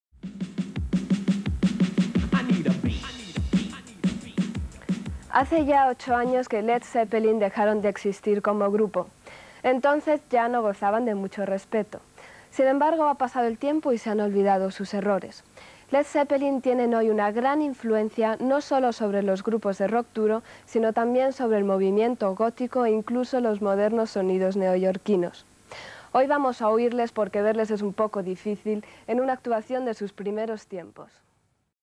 CHRISTINA ROSENVINGE presenta a LED ZEPPELIN (FM2)
FM2, 1987. Christina presentando a Led Zeppelin.
Thanks to MakAlfie!